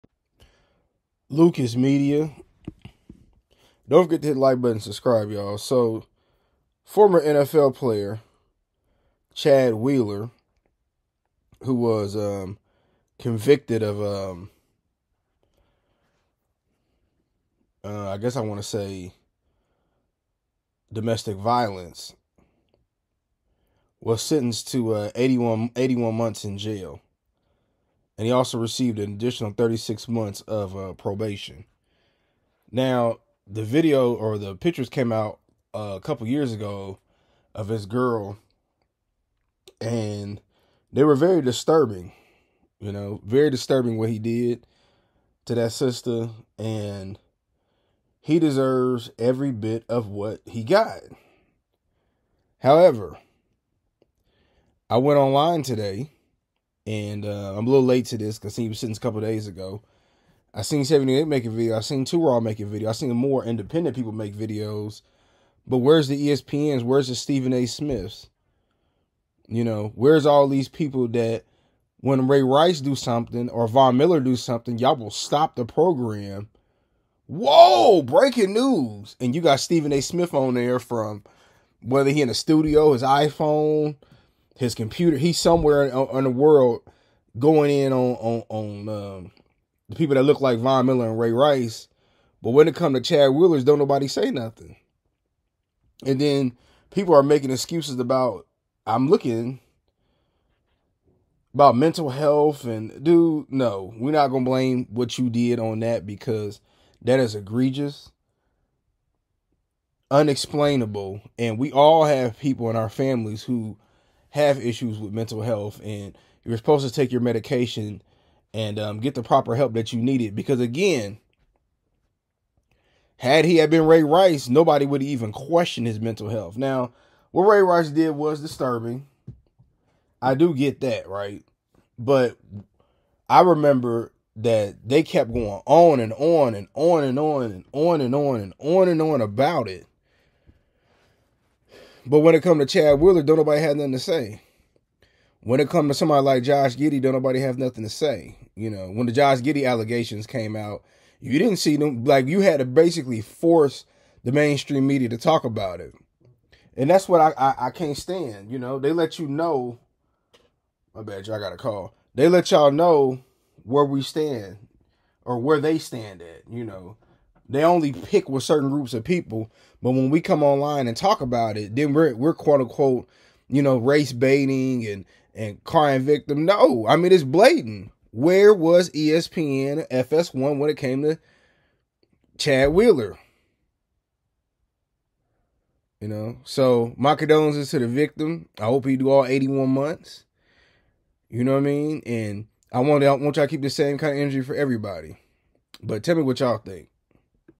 Chad Wheeler Sentenced To 81 Months In Jail
Why is major media quiet about this?